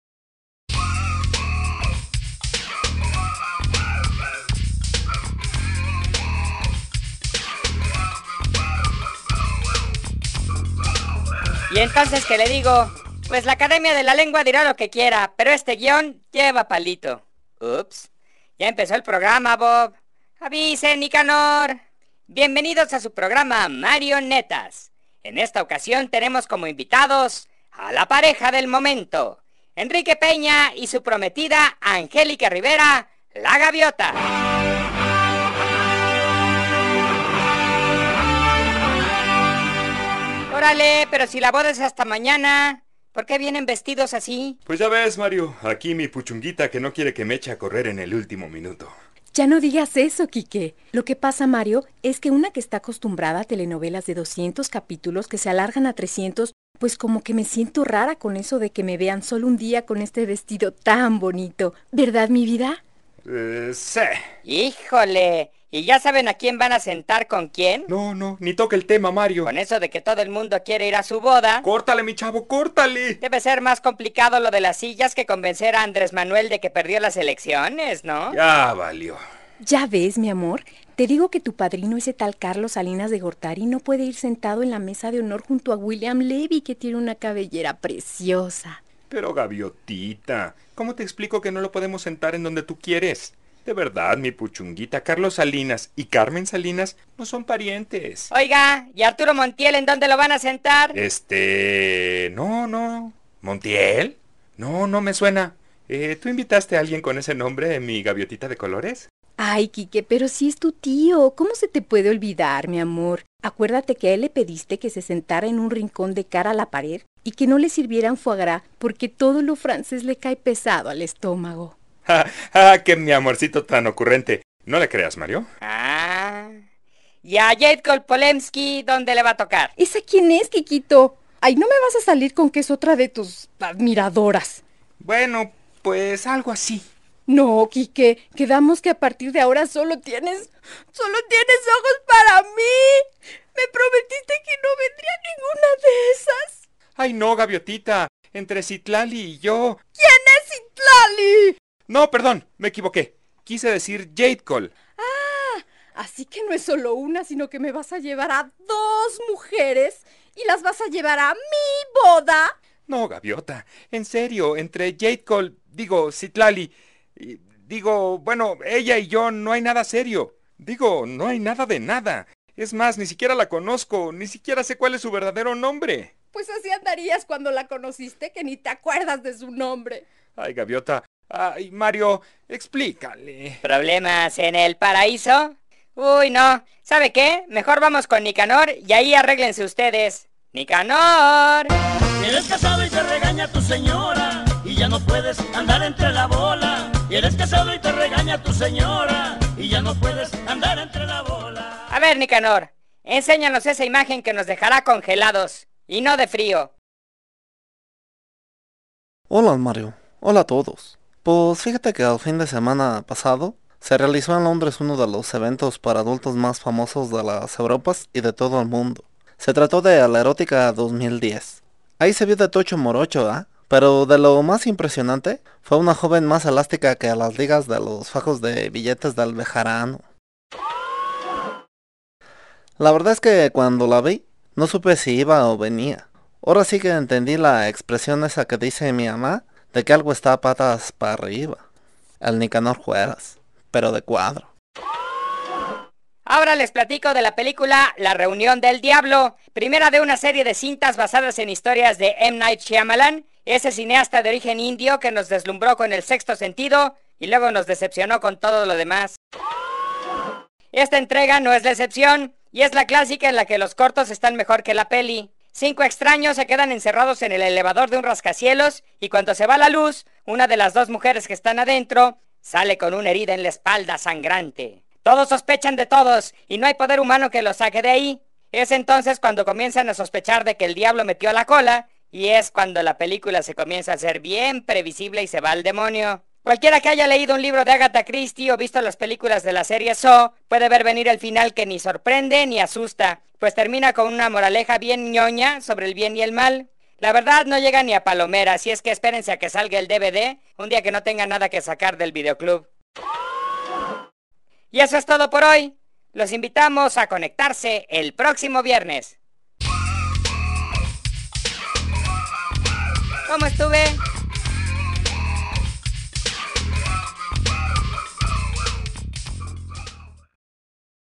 Marionetas recibe a Peña Nieto y Angélica Rivera
A unas horas de contraer matrimonio la pareja de moda llega con Mario Netas